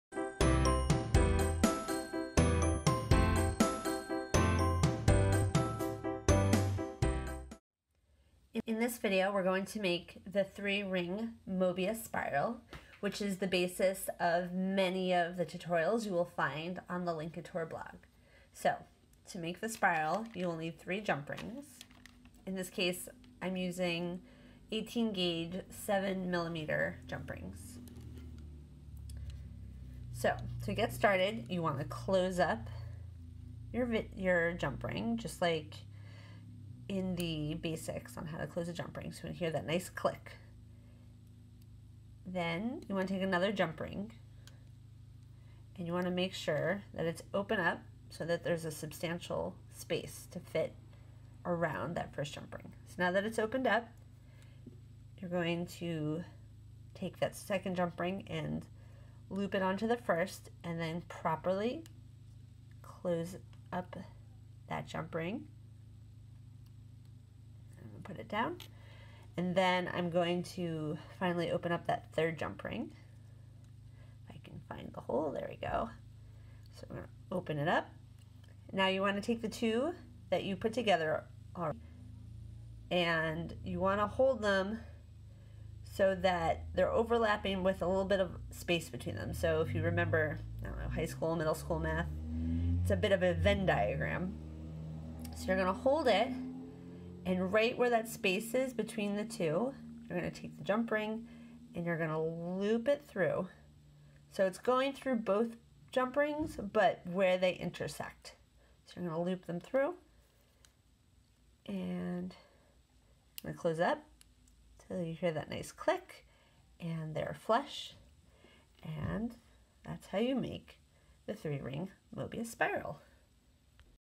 How to make a 3 ring mobius spiral
"Oh my goodness, how do you even make these spirals? Do you twist the wire around? They're so beautiful and look so complicated!"

You would not believe how many times I was asked these questions when I sold my jewelry. People were amazed by the jewelry that sparkled in the sunlight, not realizing that these seemingly complicated spirals were so unbelievably easy to make.

In this video, you'll learn the secret to those beautiful jewels. You'll learn what I've dubbed the Spiral Method. After you've made one of these, you'll have created a Mobius spiral, which you can then use to make a very simple pendant for a necklace.

This video is part of a FREE mini course course for beginners on learning how to make jump ring jewelry.